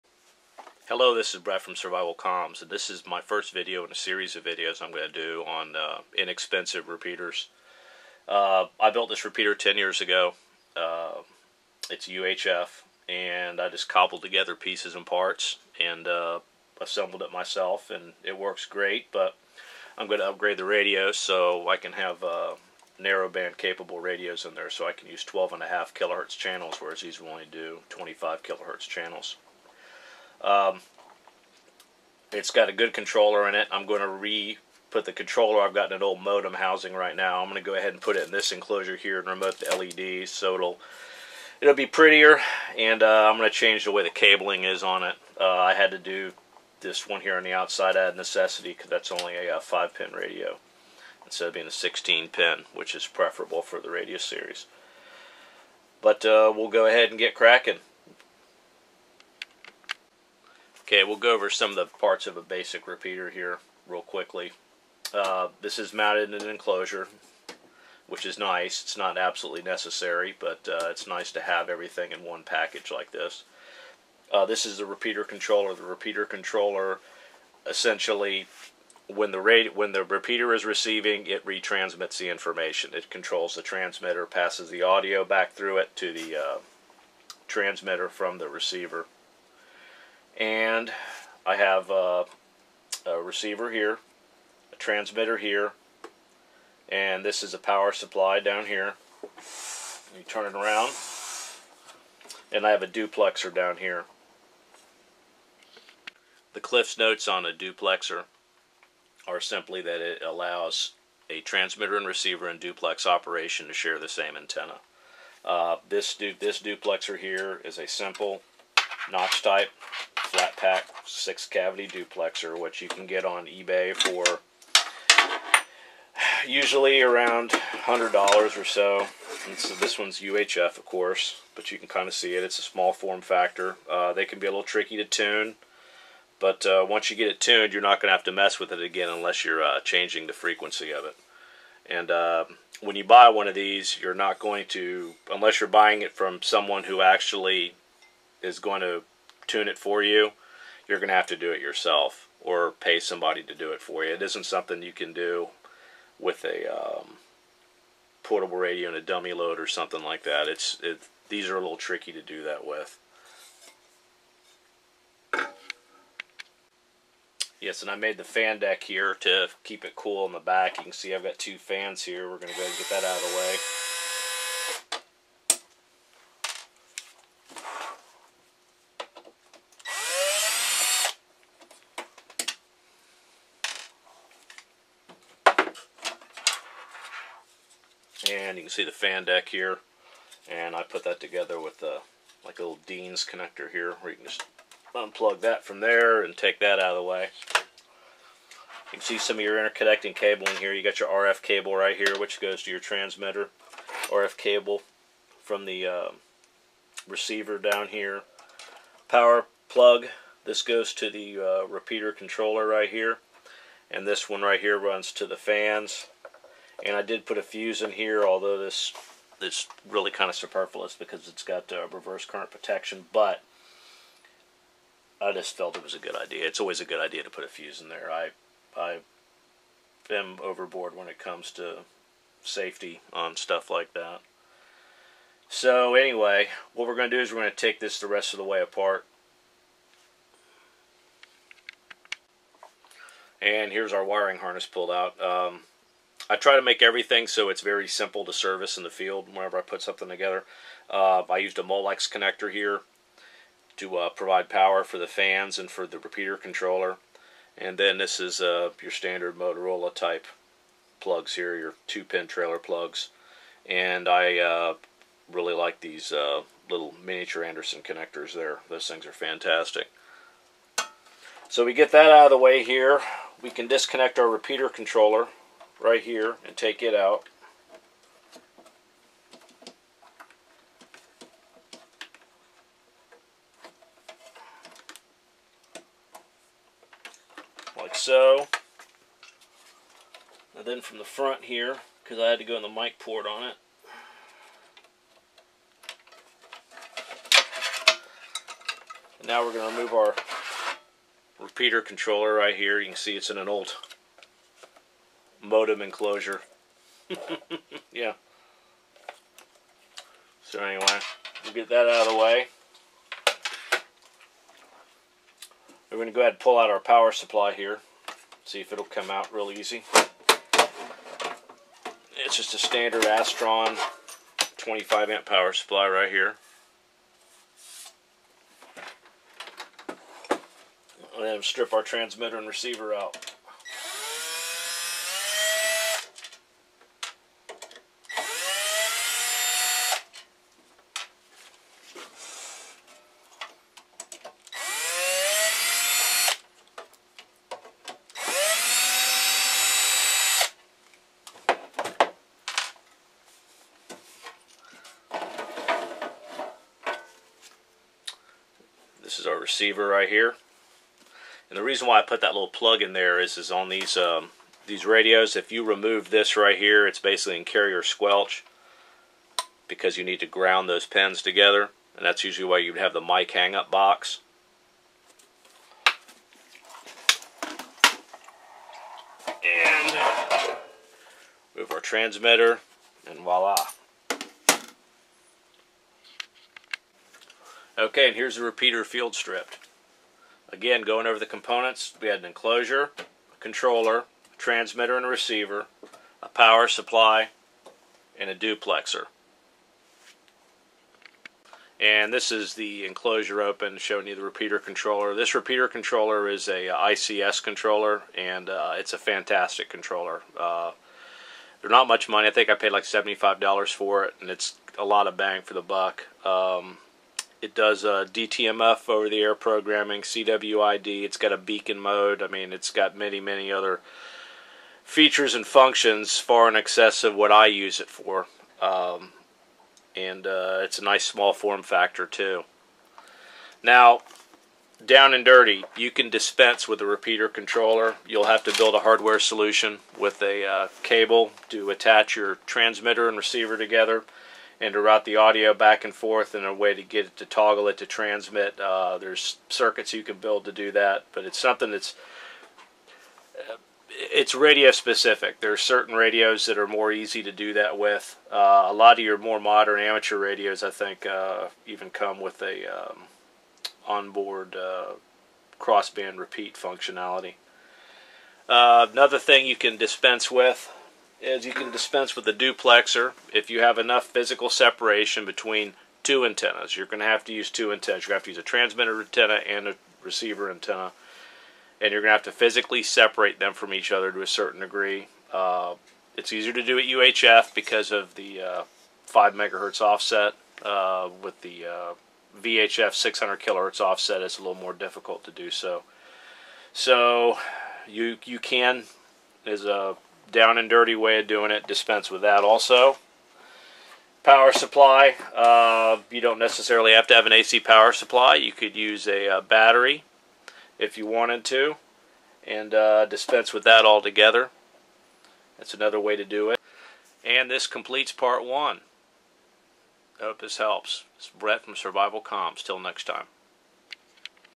Field deployable UHF repeater Part 1
Part 1 of a series of upgrading my field deployable repeater with narrowband compliant radios.  I disassemble and describe a basic UHF repeater.